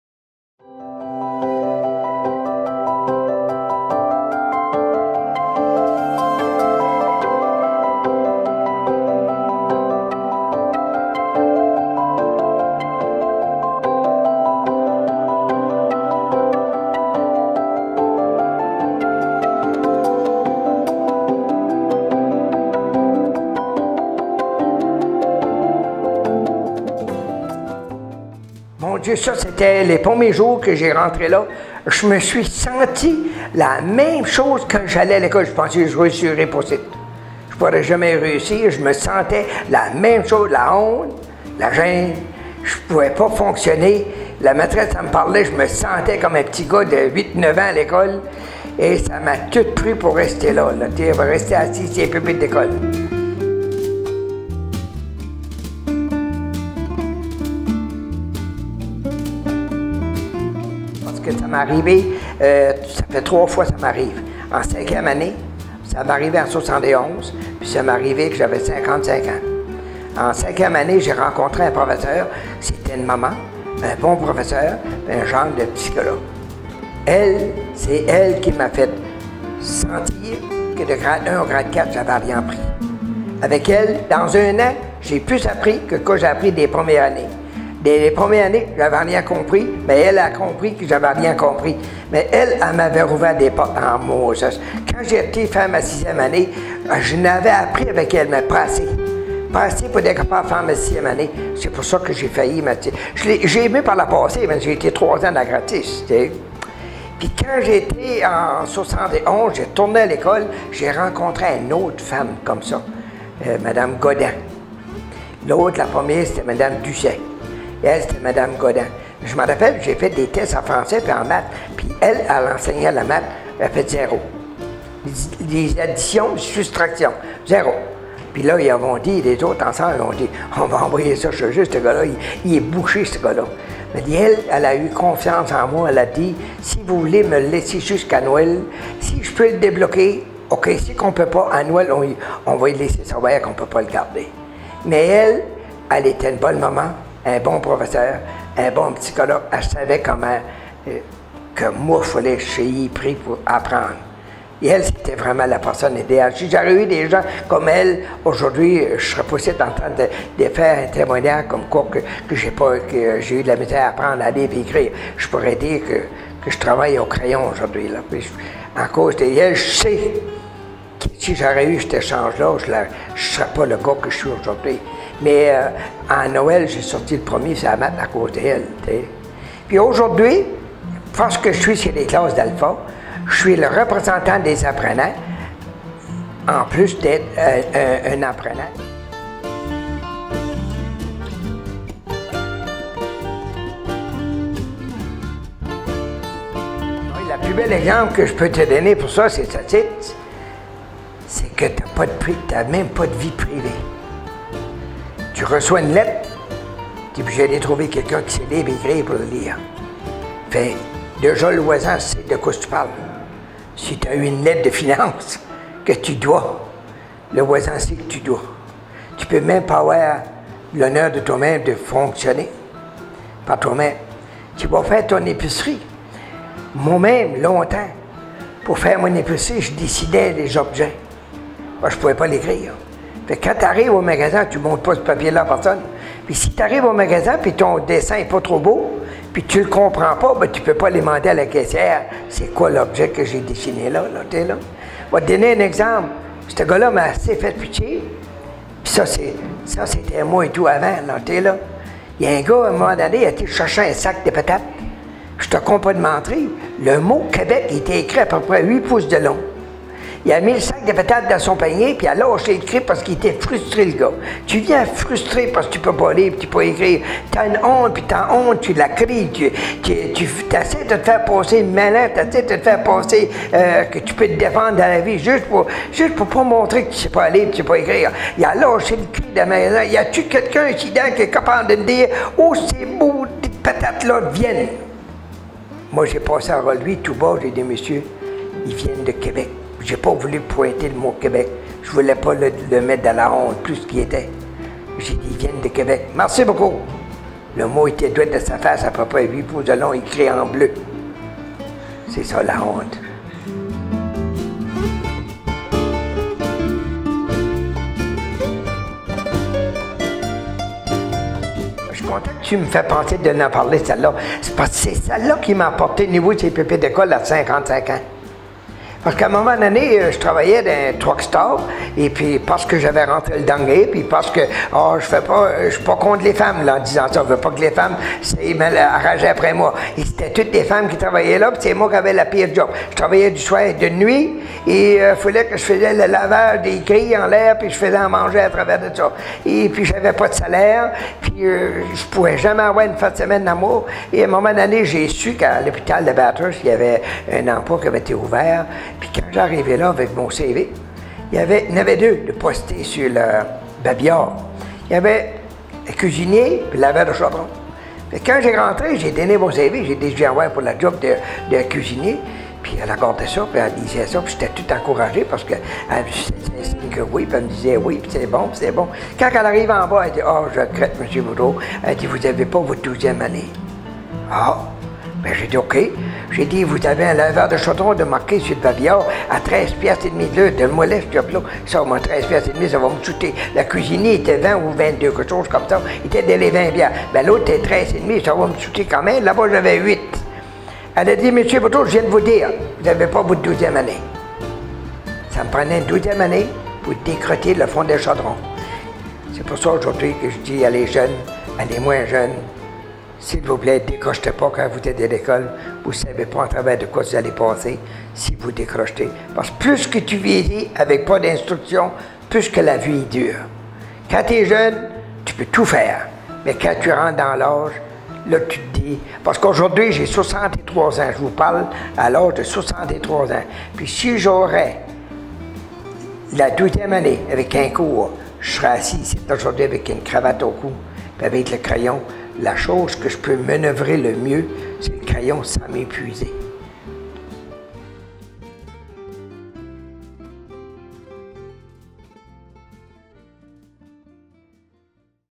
Victorin Boudreau : les obstacles au quotidien
Victorin Boudreau est représentant des personnes apprenantes pour la Fédération d'alphabétisation du Nouveau Brunswick (FANB), du Réseau pour le développement de l'alphabétisme et des compétences (RESDAC, anciennement FCAF) et du Conseil alpha de Bathurst-Chaleur (CACB).

Dans ce segment vidéo, Victorin raconte quelques anecdotes mémorables de son parcours personnel et professionnel. À travers le récit d'expériences de vie difficiles, il fait le lien avec les personnes ayant jouées un rôle significatif dans son parcours d'apprentissage et comment celles-ci ont contribué positivement à maintenir et enrichir son estime personnelle tout au long de sa vie.